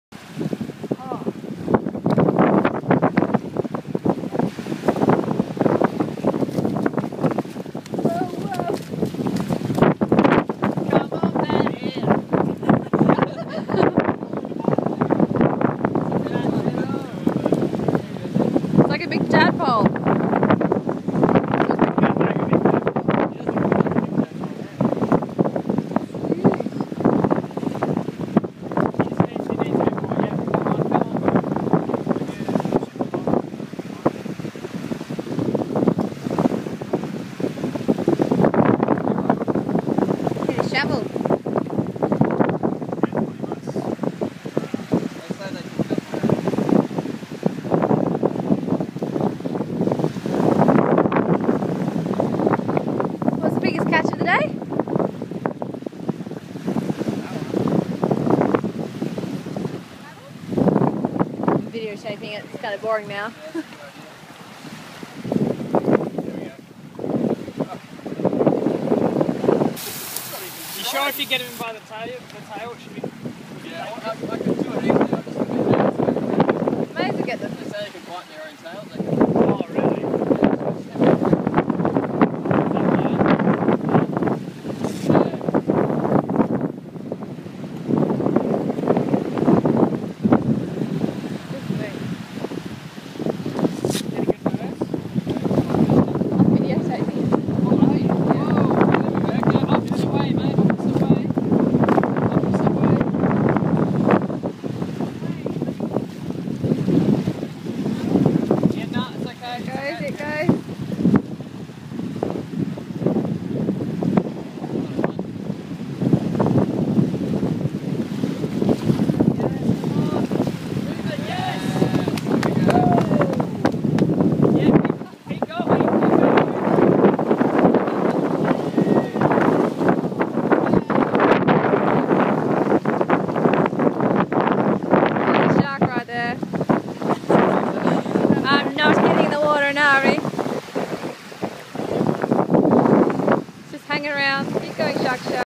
"Comm'on Fatty" Shark Released Rotto 2014
A wobbegong shark got trapped in our cray pot on Australia Day.  We released her but due to her being pregnant it took her a while to wriggle back into the water.